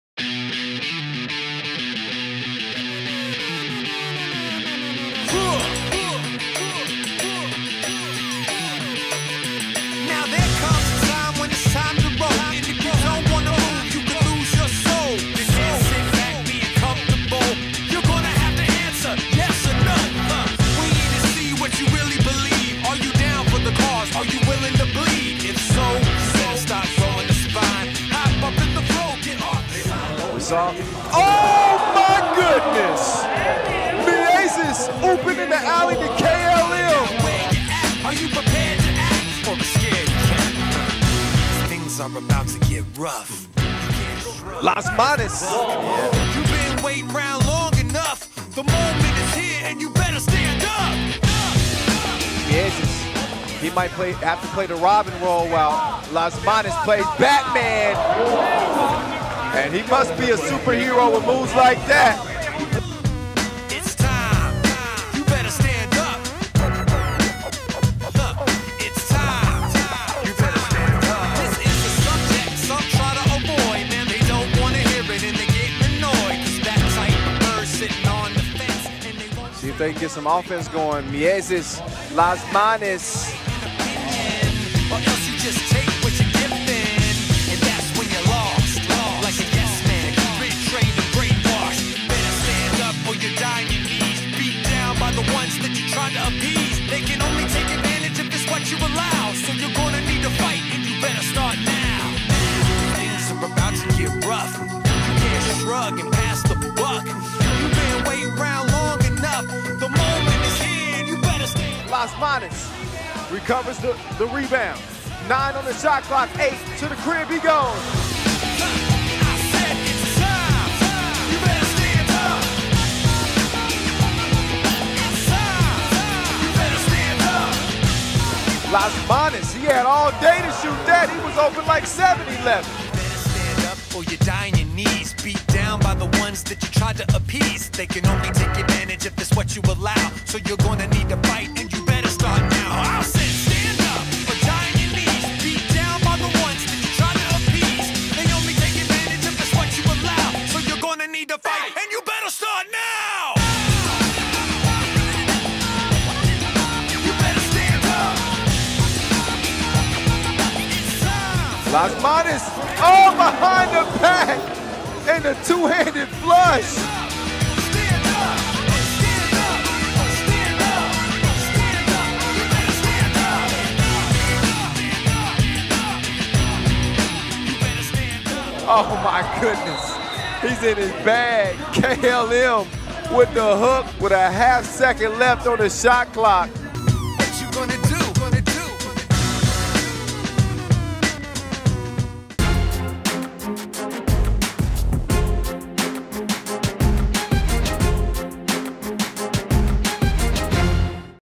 Karlis Lasmanis - Riga | MSP Mixtape | FIBA 3x3 World Tour 2018 - Chengdu Masters
Enjoy this Most Spectacular Player Mixtape featuring Riga's Karlis Lasmanis from the FIBA 3x3 World Tour 2018 - Chengdu Masters.

Keep up-to-date with the 3x3WT right here on YouTube!